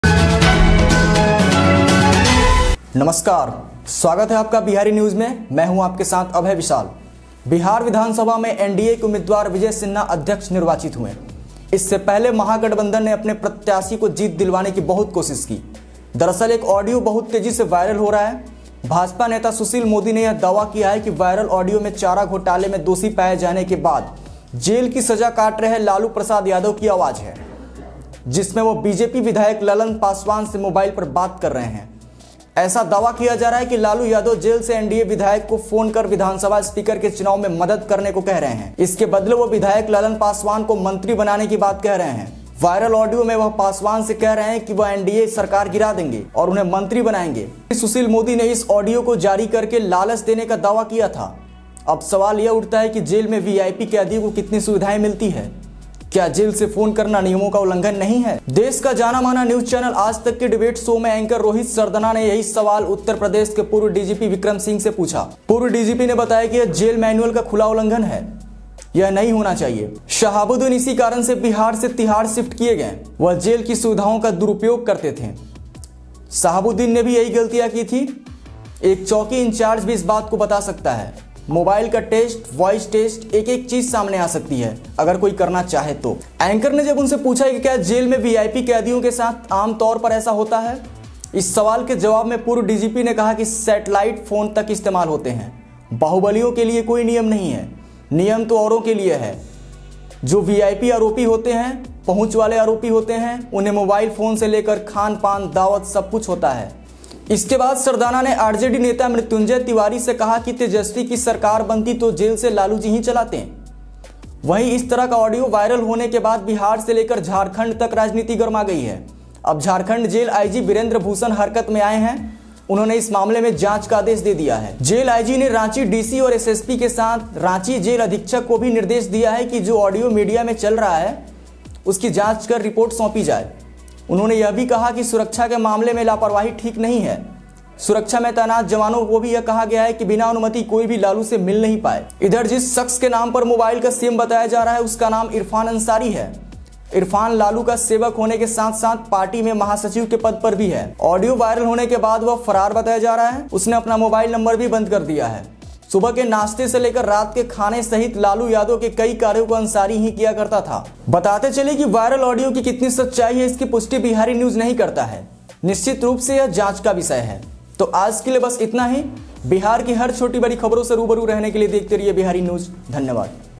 वायरल ऑडियो ने मचाया बवाल, जानिए किसने क्या कहा ?जेल आईजी ने दिए जांच के आदेश
वायरल ऑडियो ने मचाया बवाल, जानिए किसने क्या कहा ?
जेल आईजी ने दिए जांच के आदेश